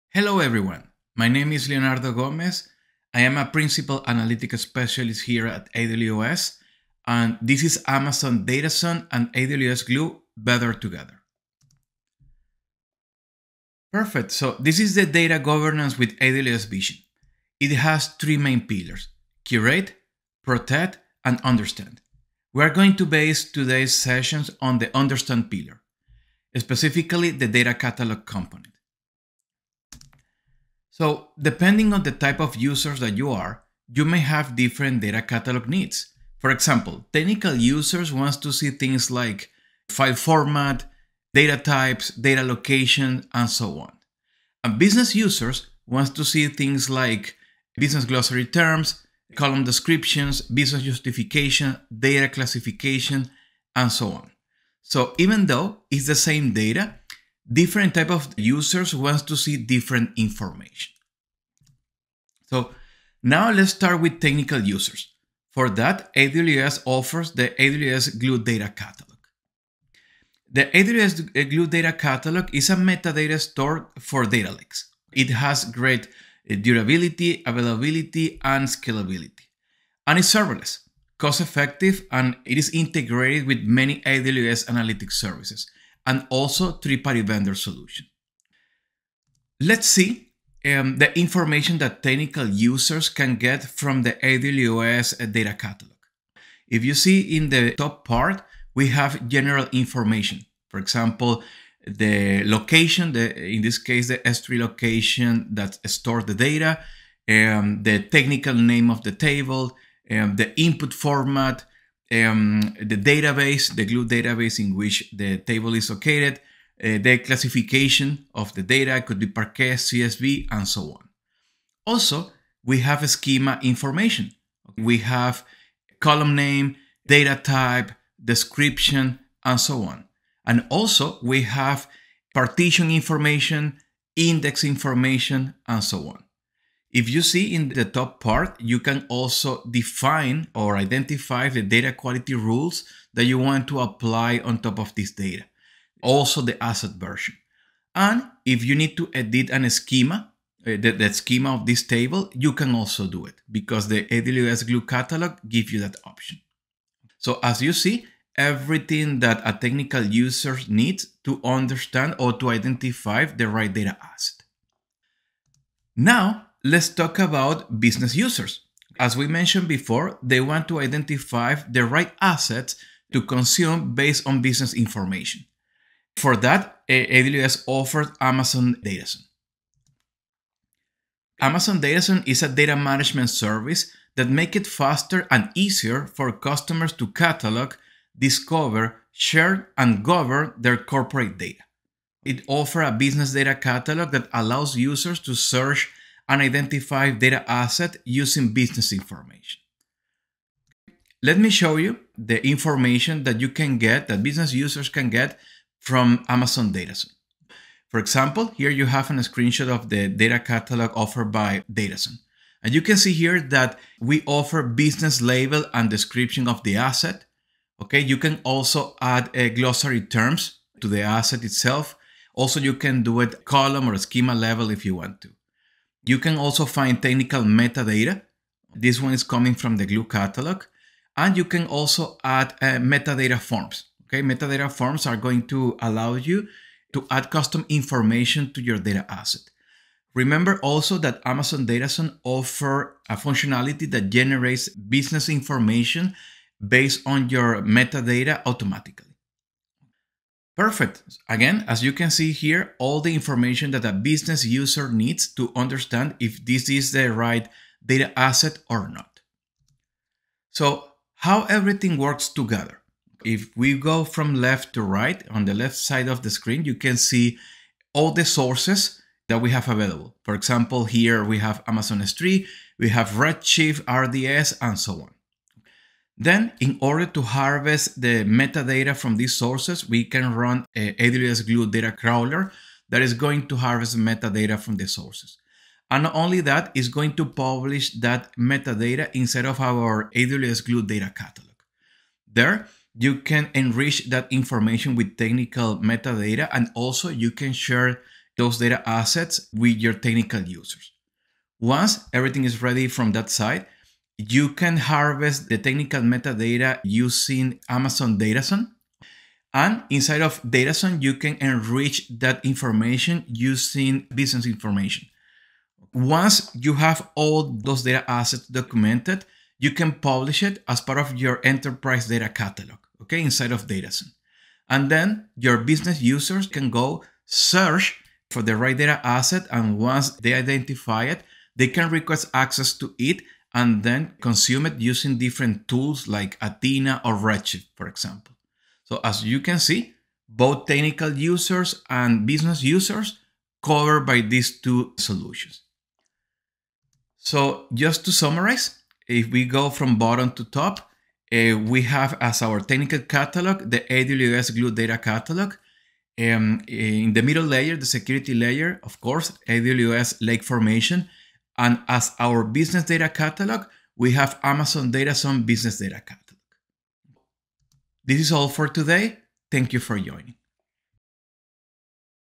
Build a data catalog solution for both business and technical users with Amazon DataZone & AWS Glue
Learn how to manage a technical metadata catalog that integrates with a business data catalog. AWS solutions include AWS Glue Data Catalog and Amazon DataZone.